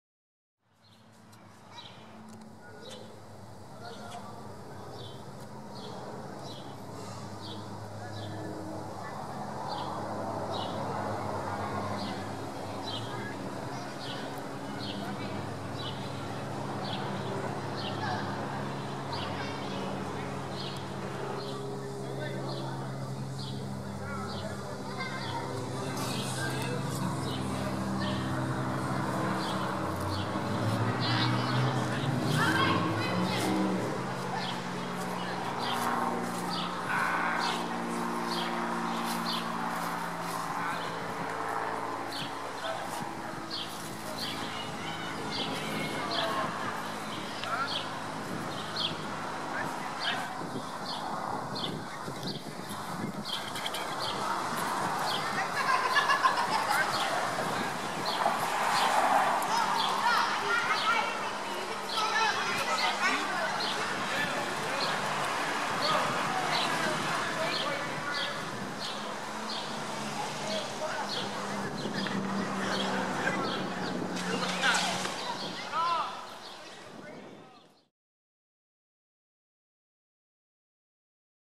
2014 07 11 FairFoodsButterfly
A guided tour of Judy's garden in Dorchester.